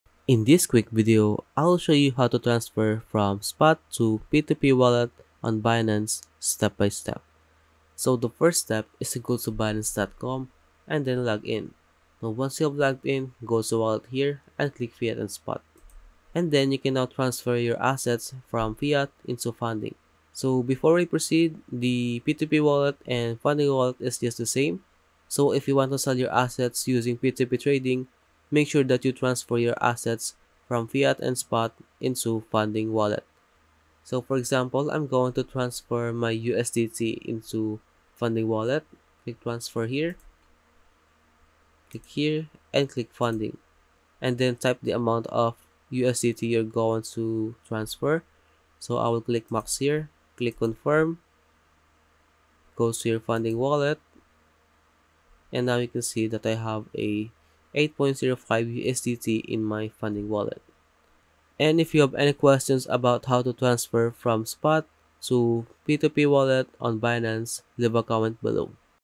How to Transfer from Spot to P2P Wallet on Binance (Step by Step)
In this quick video, I will show you How to Transfer from Spot to P2p Wallet on Binance step by step.

Welcome to Binance, one of the leading cryptocurrency exchanges in the world. In this video, we'll explore how to transfer funds between your Spot and P2P wallets on Binance.

To get started, let's suppose you have some funds in your P2P wallet that you want to transfer to your Spot wallet.

First, go to your P2P wallet and select the currency you wish to transfer.

Next, click "Withdraw" and enter the amount you wish to transfer.

Make sure you select "Binance Spot Wallet" as the destination wallet.

Once you've entered all the required information, hit "Submit" to initiate the transfer.

Depending on the currency you're transferring, the transfer may take some time to process. But once it's complete, you should see the funds available in your Spot wallet.

Now suppose you want to transfer funds the other way - from your Spot wallet to your P2P wallet.

To do this, go to your Spot wallet and select the currency you wish to transfer.

Then click "Deposit" and select "Binance P2P Wallet" as the destination wallet.

Next, enter the amount you wish to transfer and hit "Submit."

Again, the transfer may take some time to process, but once it's complete, you should see the funds available in your P2P wallet.

And that's it! You now know how to transfer funds between your Spot and P2P wallets on Binance.